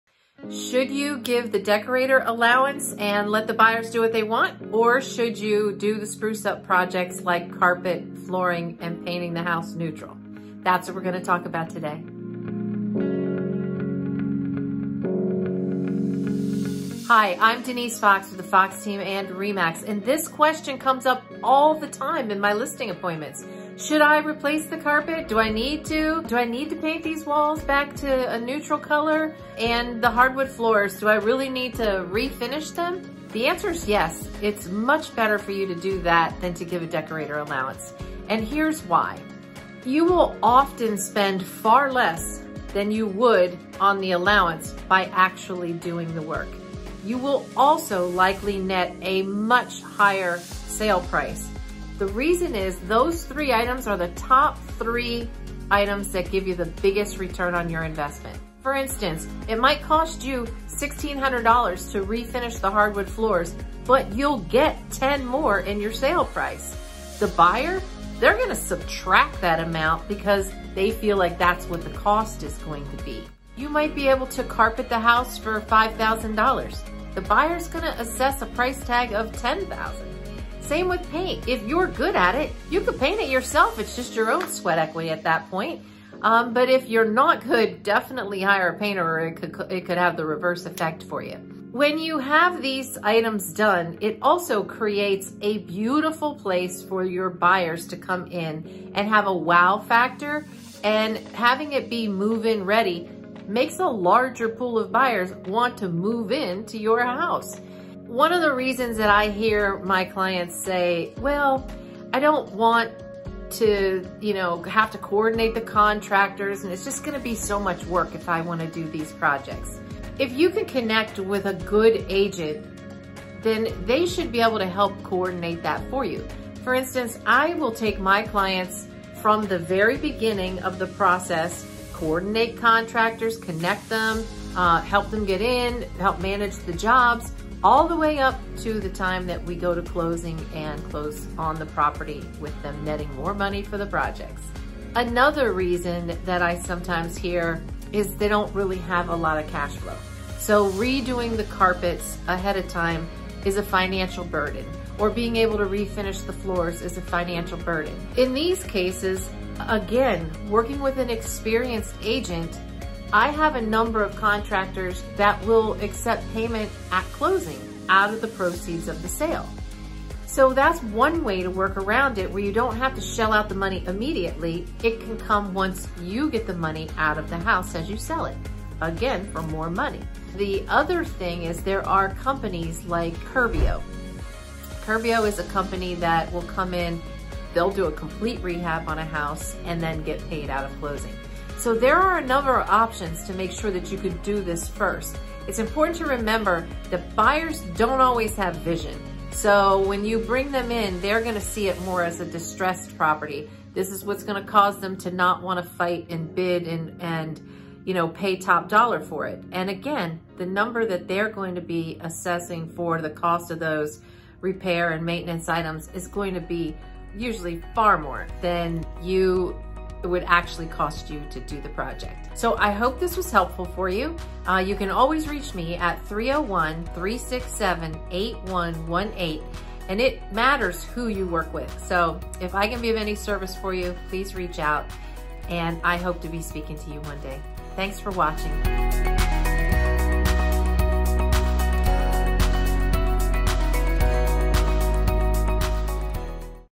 Do you do the spruce projects (flooring, carpet & paint) or give a decorating/carpet allowance?
Should you give the decorator allowance and let the buyers do what they want, or should you do the spruce-up projects like carpet, flooring and painting the house neutral? That's what we're going to talk about today.

Hi, I'm Denise Fox for the Fox Team and RE/MAX.

This question comes up all the time in my listing appointments. Should I replace the carpet? Do I need to? Do I need to paint these walls back to a neutral color? And the hardwood floors, do I really need to refinish them? The answer is yes. It's much better for you to do that than to give a decorator allowance, and here's why. You will often spend far less than you would on the allowance by actually doing the work. You will also likely net a much higher sale price. The reason is those three items are the top three items that give you the biggest return on your investment.

For instance, it might cost you $1,600 to refinish the hardwood floors, but you'll get 10 more in your sale price. The buyer; they're going to subtract that amount because they feel like that's what the cost is going to be. You might be able to carpet the house for $5,000. The buyer's going to assess a price tag of 10,000. Same with paint. If you're good at it, you could paint it yourself. It's just your own sweat equity at that point. But if you're not good, definitely hire a painter; it could have the reverse effect for you. When you have these items done, it also creates a beautiful place for your buyers to come in and have a wow factor, and having it be move-in ready makes a larger pool of buyers want to move into your house.

One of the reasons I hear my clients say, "Well, I don't want to have to coordinate the contractors. It's just going to be so much work if I want to do these projects." If you can connect with a good agent, then they should be able to help coordinate that for you. For instance, I will take my clients from the very beginning of the process, coordinate contractors, connect them, help them get in, help manage the jobs all the way up to the time that we go to closing and close on the property with them netting more money for the projects.

Another reason that I sometimes hear is they don't really have a lot of cashflow. So redoing the carpets ahead of time is a financial burden or being able to refinish the floors is a financial burden. In these cases, again, working with an experienced agent, I have a number of contractors that will accept payment at closing out of the proceeds of the sale. So that's one way to work around it, where you don't have to shell out the money immediately; it can come once you get the money out of the house as you sell it, again, for more money.

The other thing is there are companies like Curbio. Curbio is a company that will come in, they'll do a complete rehab on a house and then get paid out of closing. So there are a number of options to make sure that you could do this first.

It's important to remember that buyers don't always have vision. So when you bring them in, they're going to see it more as a distressed property. This is what's going to cause them to not want to fight and bid and pay top dollar for it. Again, the number that they're going to be assessing for the cost of those repair and maintenance items is going to be usually far more than it would actually cost you to do the project.

I hope this was helpful for you. It matters who you work with. If I can be of any service for you, please reach out. I hope to be speaking to you one day.